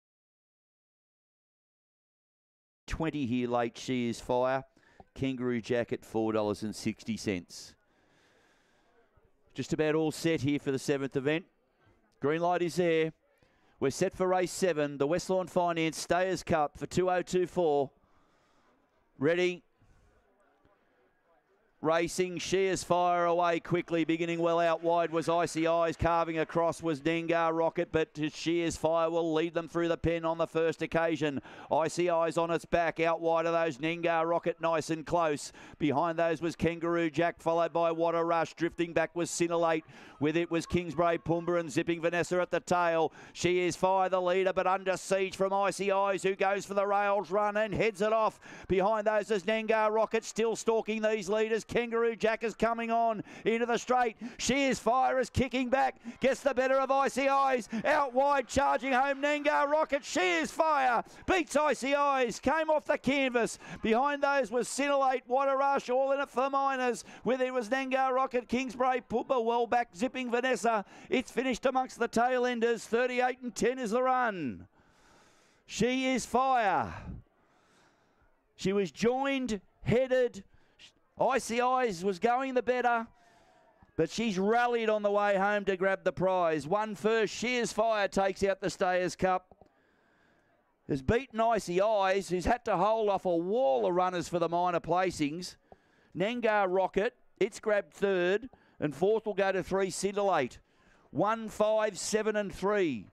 2024 Grafton Stayers Cup
The Mark Moroney Trained She Is Fire Wins The Grafton Stayers Cup From Box 4 At Grafton GRC Last Night.